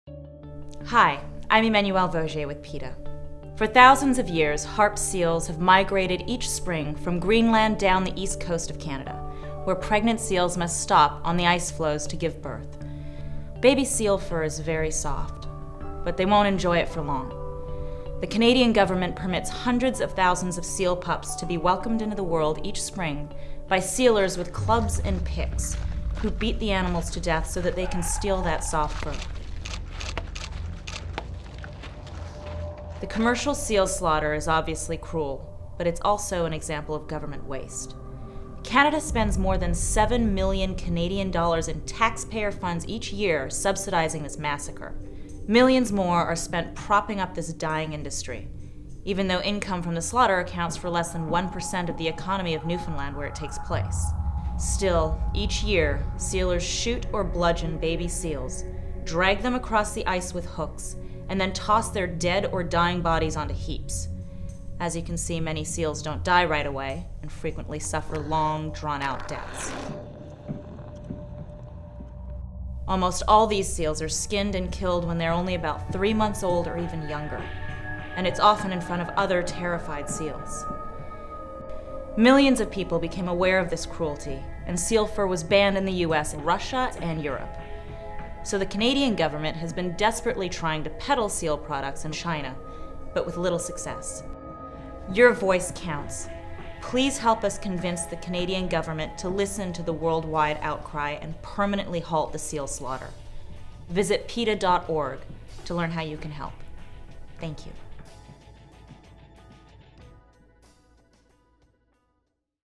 Emmanuelle Vaugier: Canadian Seal Slaughter
Canadian actor Emmanuelle Vaugier exposes Canada's gruesome seal slaughter and encourages fans to help stop the annual massacre today.